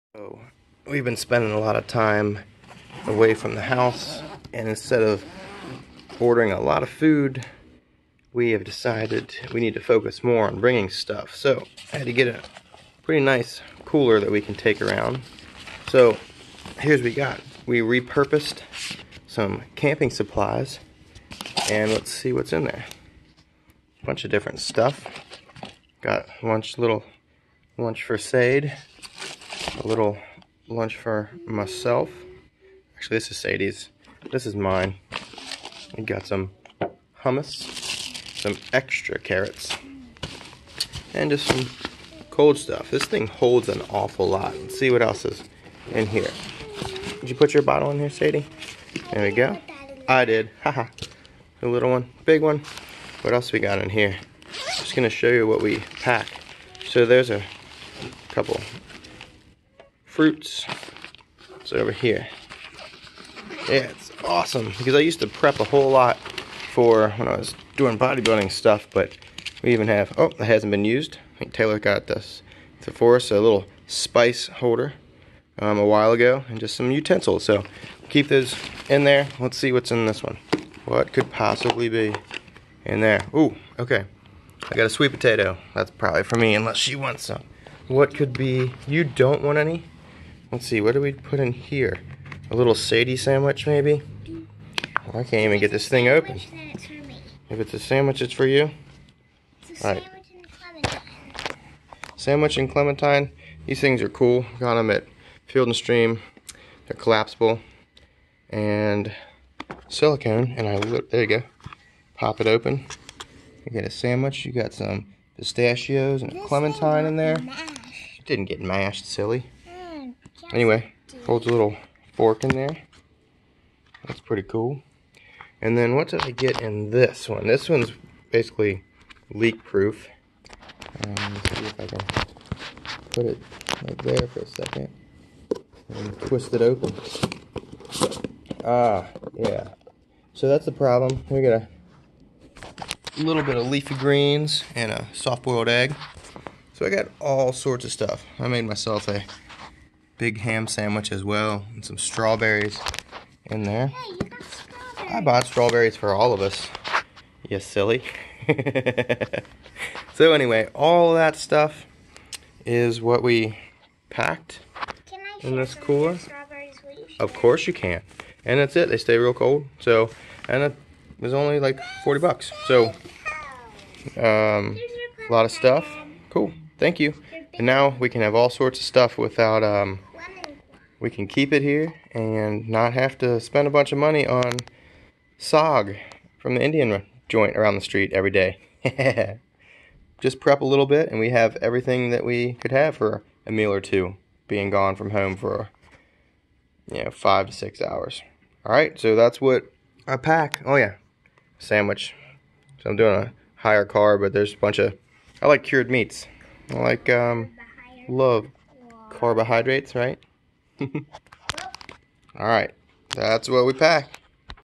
What Foods Do I Pack for a Day at Work? (and for my kid ! )
Its not that hard to pack foods for a day at work. Just focus on easy to clean storage and lots of micronutrient rich foods. It shouldnt be stressful.